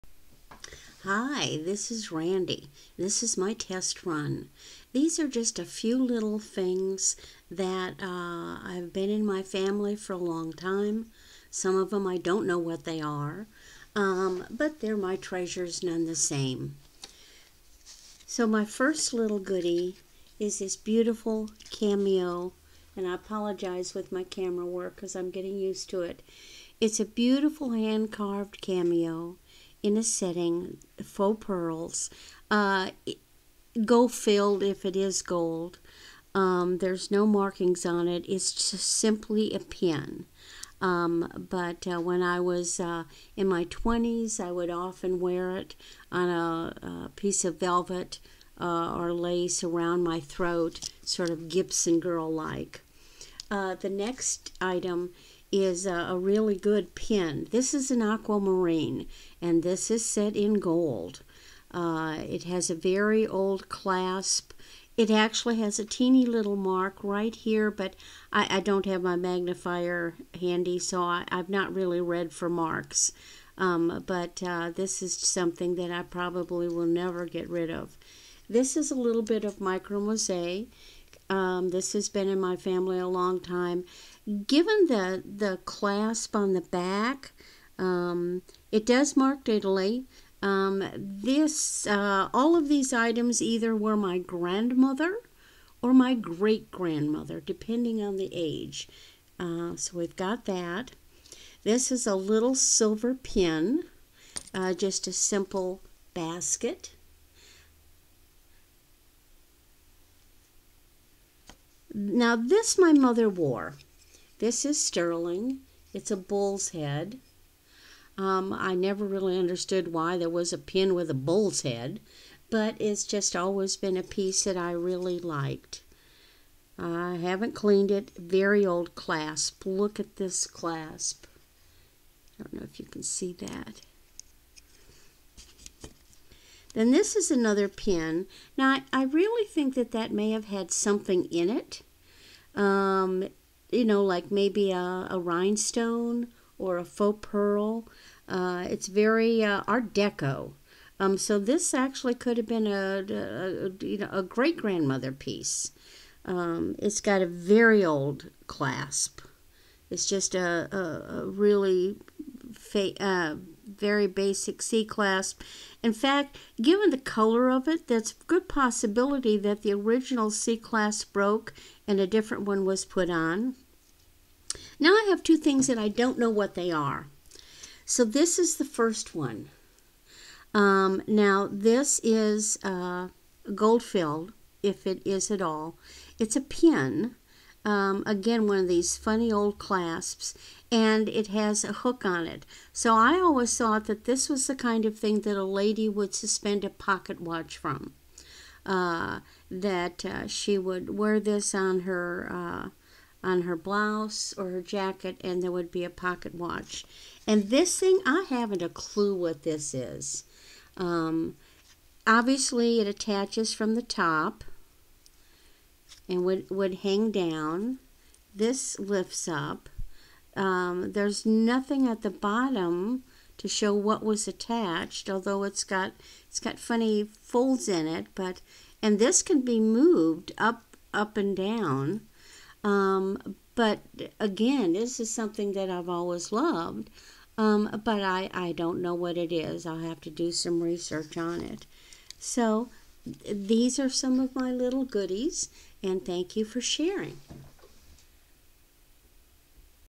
My vintage Treasures 12 30 23
My first test run showing some family vintage jewelry items